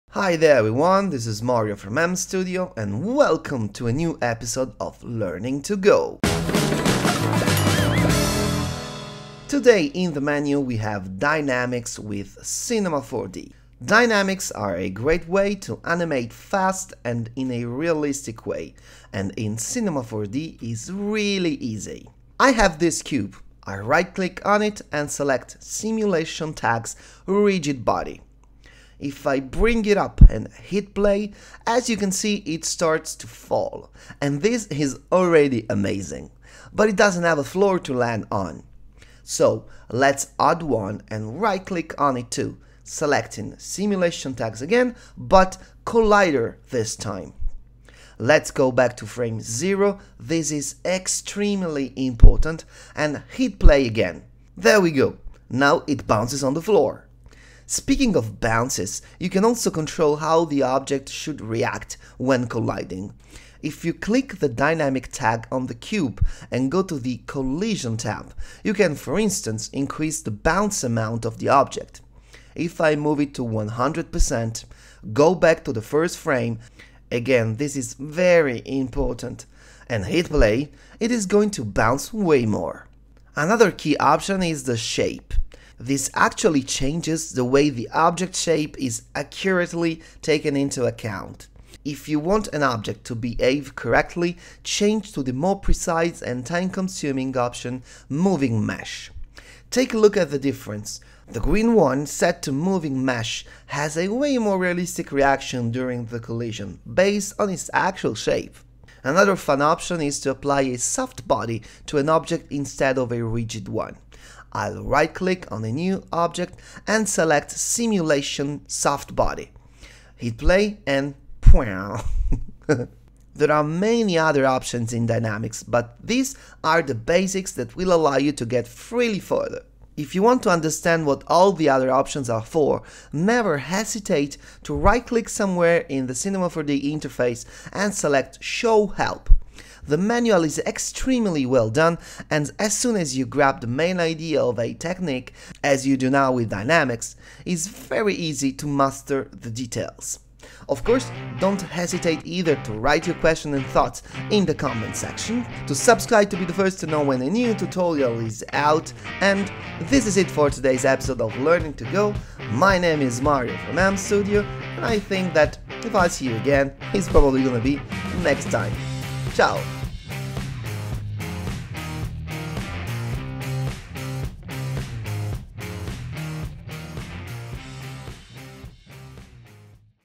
Learn Dynamics in Cinema 4D in 3 minutes
DID YOU ENJOY THE TUTORIAL? WANT TO TIP THE TRAINER? EVEN 1 CENT IS OK!

Dynamics are the easiest and most powerful technique to realistically animate objects. Falling, hitting each other, bouncing around and more. Learn the basics of this workflow in only 3 minutes in my new tutorial!